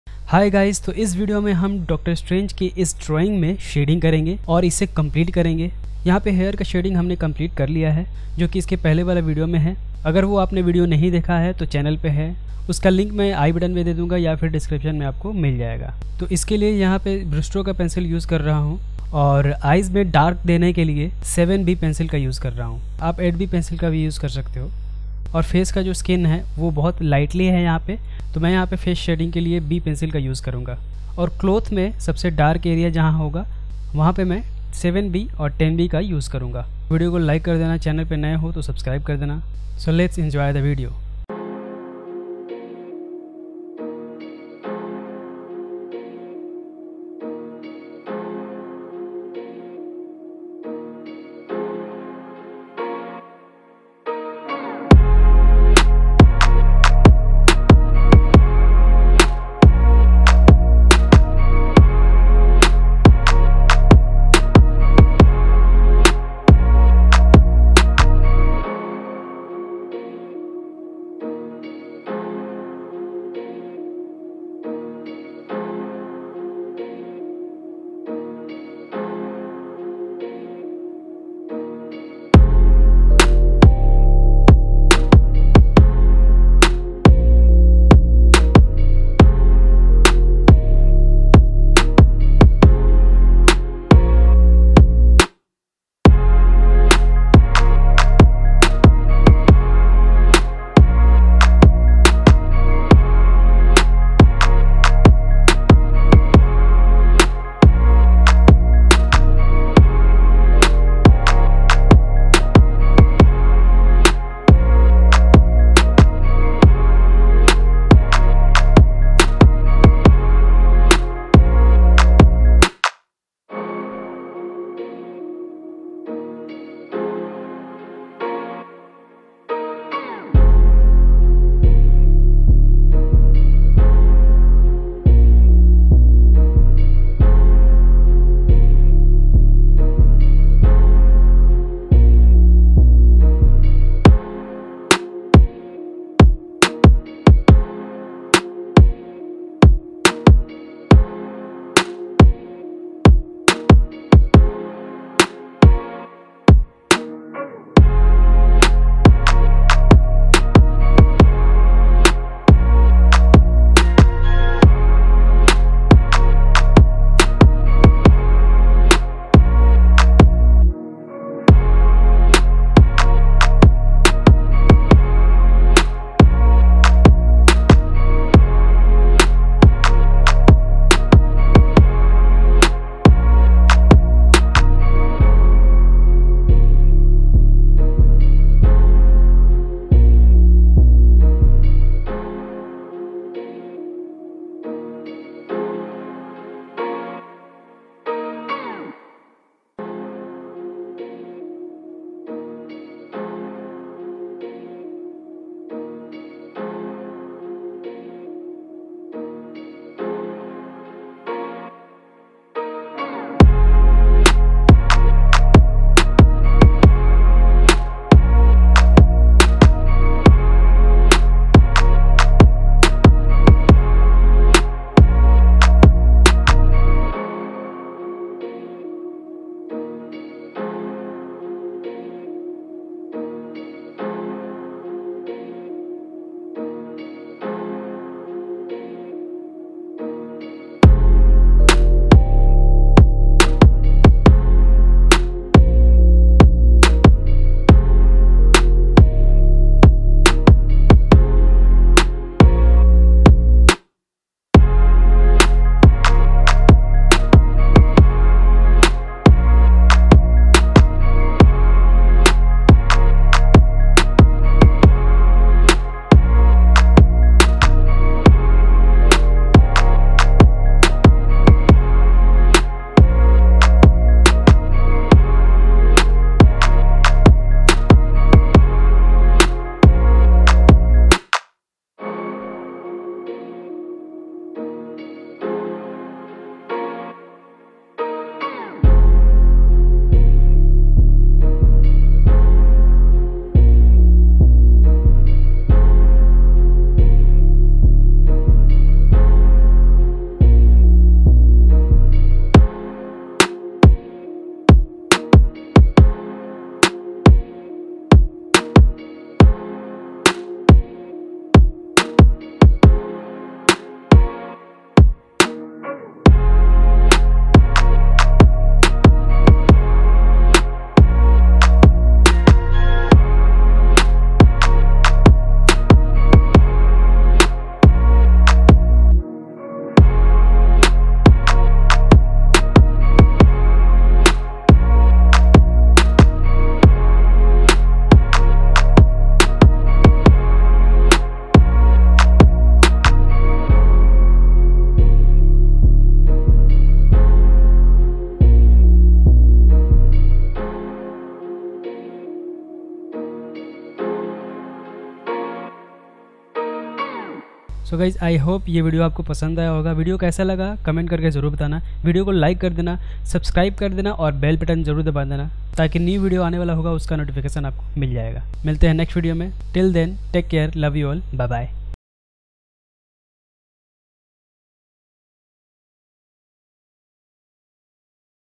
Doctor Strange Pencil Drawing | How To Draw Portrait with Shading
Hi guys,
This video is all about How To Draw Realistic Portrait of Doctor Strange.In this video we will complete the drawing with shading. I hope you enjoy and love this video.